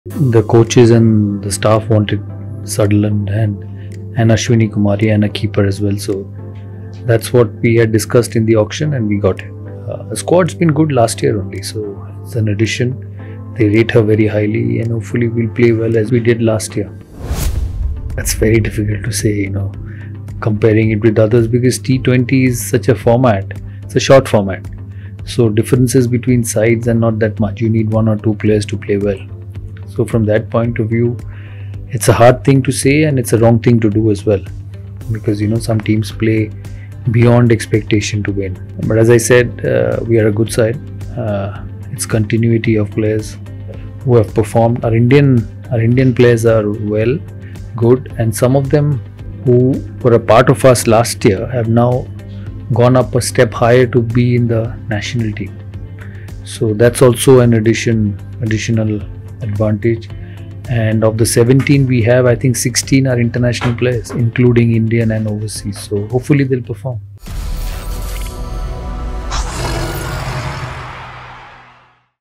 𝑺𝒕𝒓𝒐𝒏𝒈𝒆𝒓 𝒂𝒏𝒅 𝒃𝒆𝒕𝒕𝒆𝒓 💪👊
📹| Listen in to our Director of Cricket Sourav Ganguly on our Class of 2024 🙌